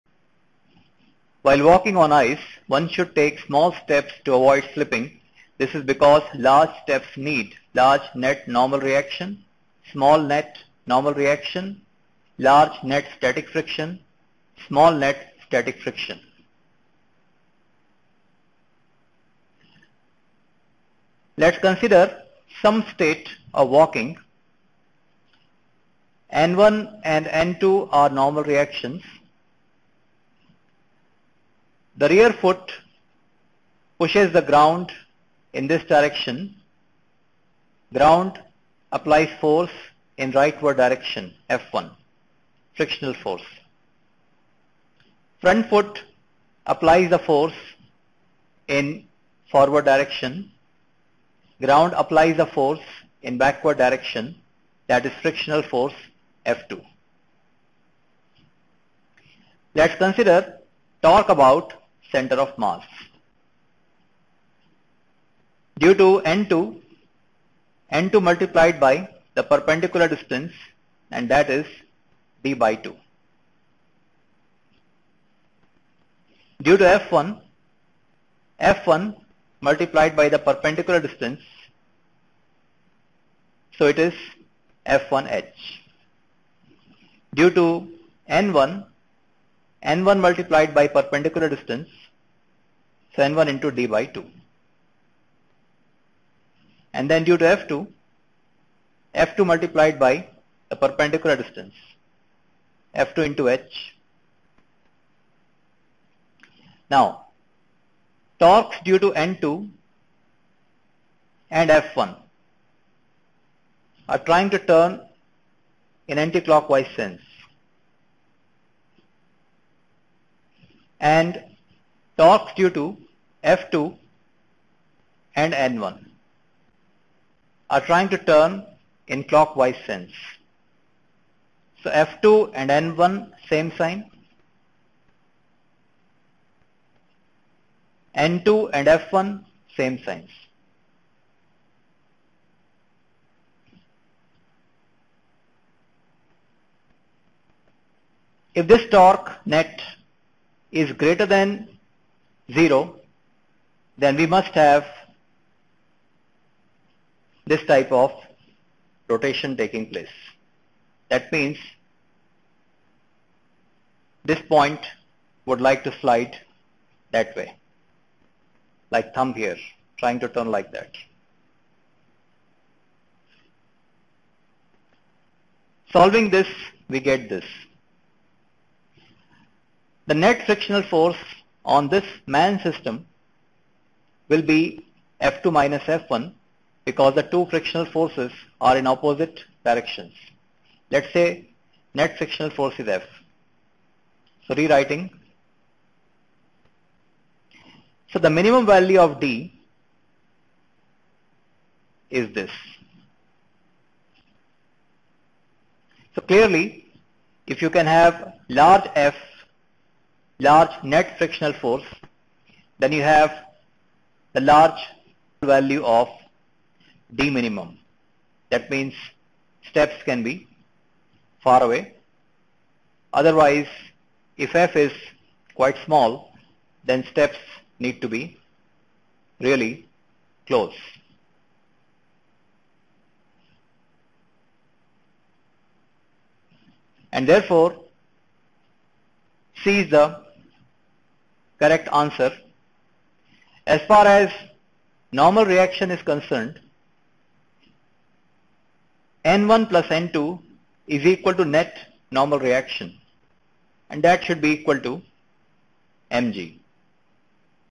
Physics of Walking, Walking on Ice
Physics of walking is explained by Mr. Manish Verma taking a question involving walking on ice considering one particular state of walking. Step size is particularly important in case of walking on ice as the coefficient of friction is quite small in this case.